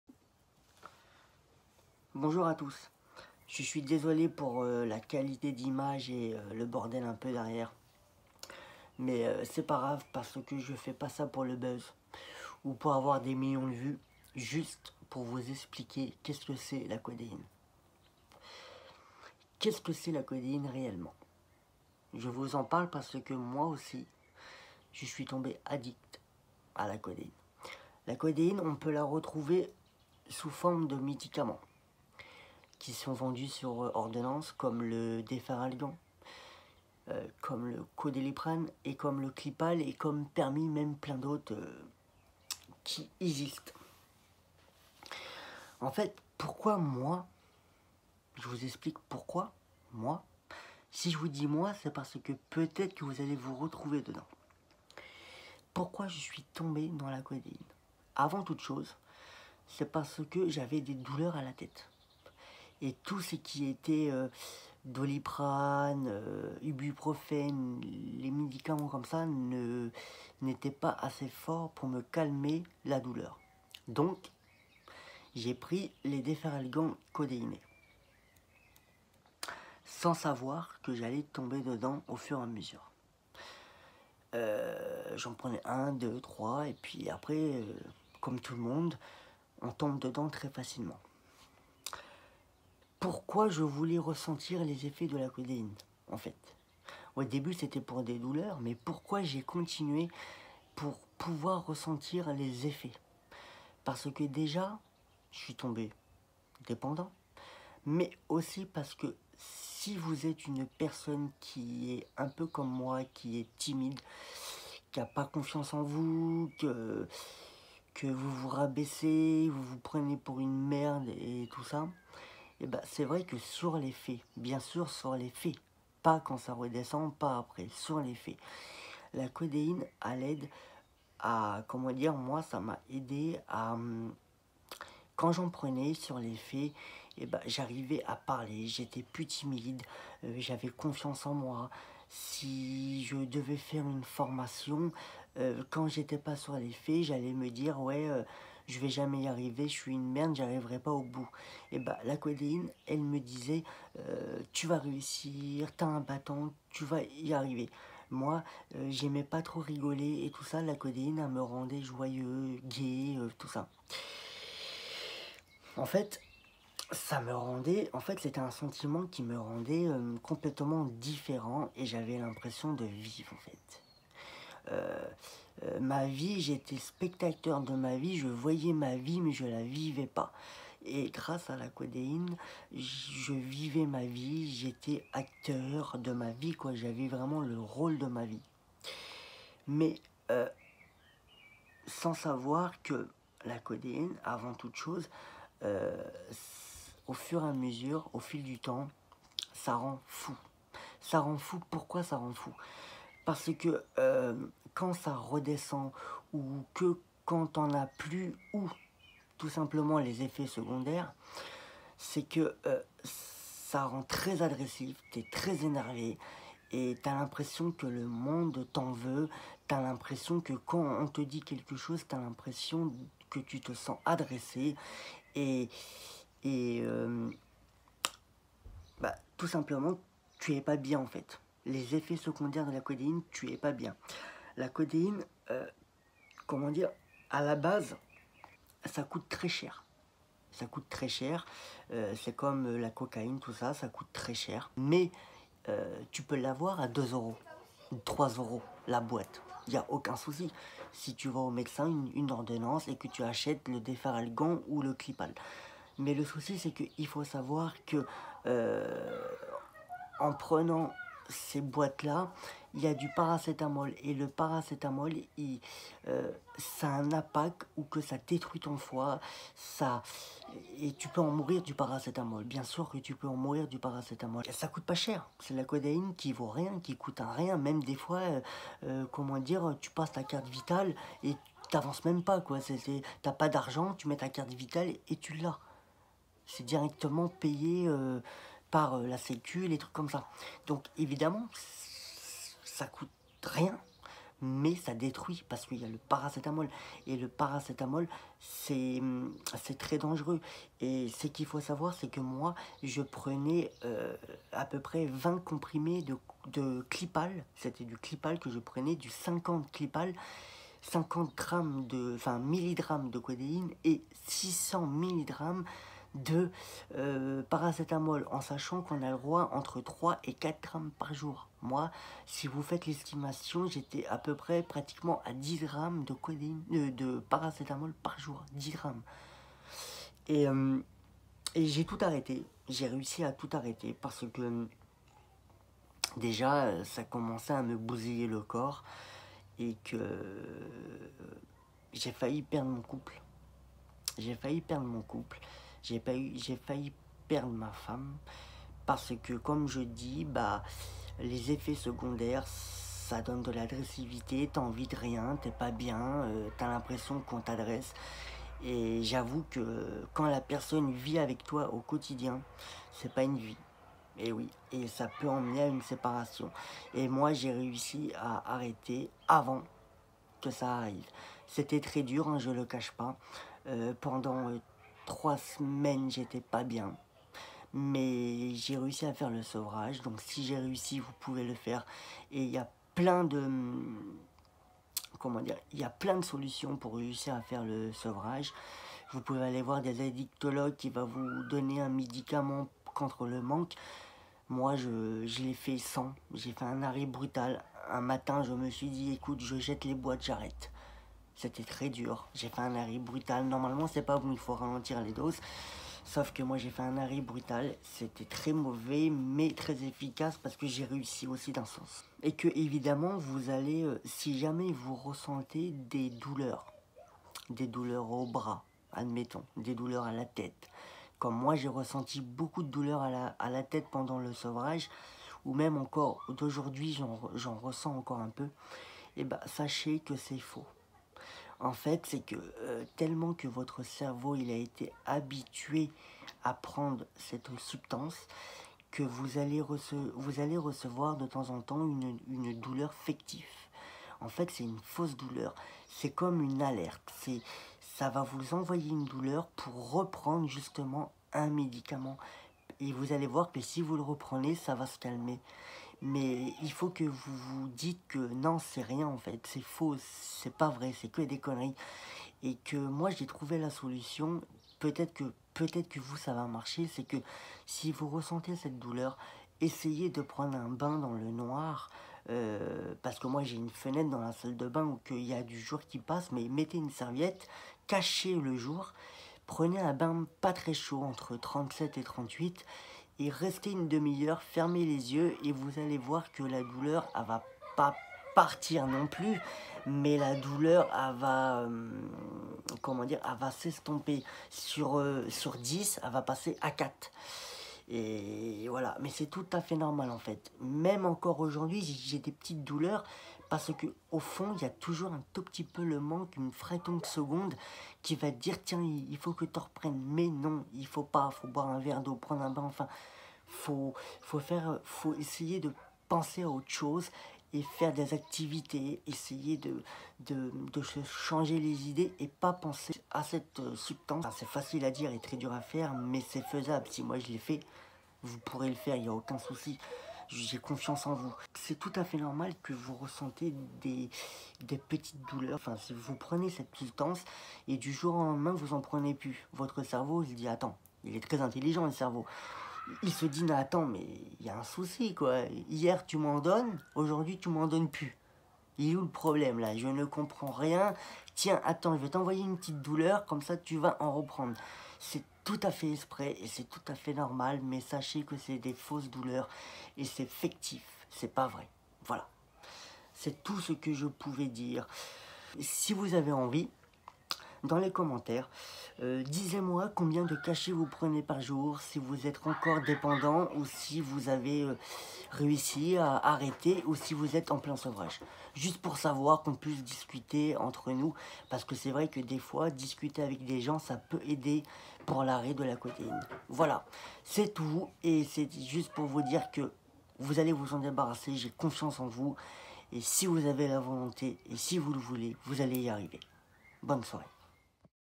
Addict à la codéine j’ai arrêté 💊
Dans cette vidéo vous allez voir comment j’ai réussi à arrêter la dépendance à la codéine quelques conseils que vous pouvez faire et surtout les dangers